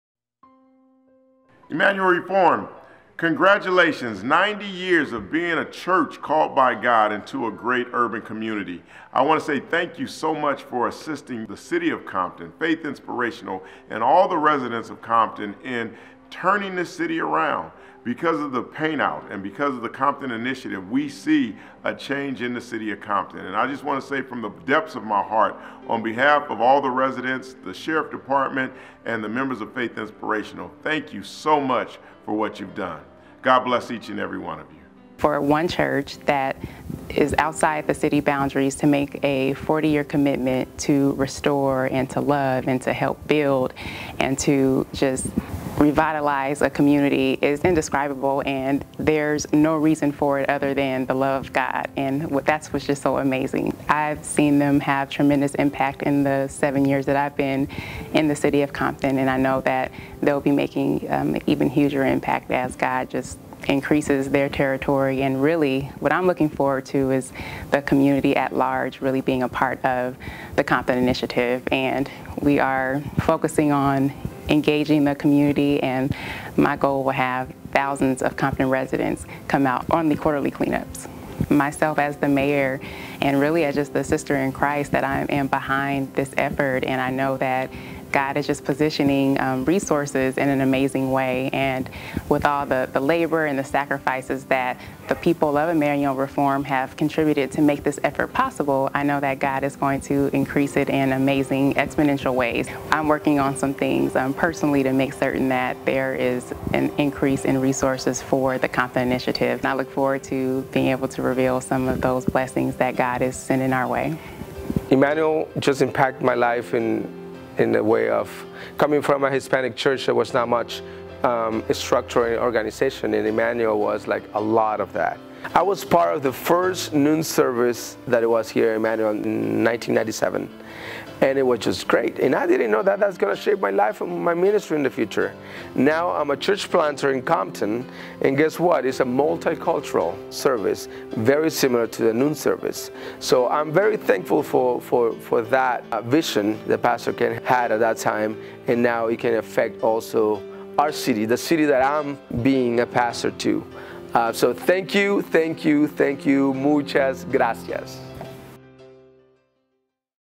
Emmanuel Reformed Church - Making a Difference in the City of Compton
Pastor and Sheriff Rafer Owens, Mayor Aja Brown, and Church Planter Arnoldo Escobar all share about the impact that Emmanuel has had in the city of Compton, and the hopes and dreams for the future of transformation in that city.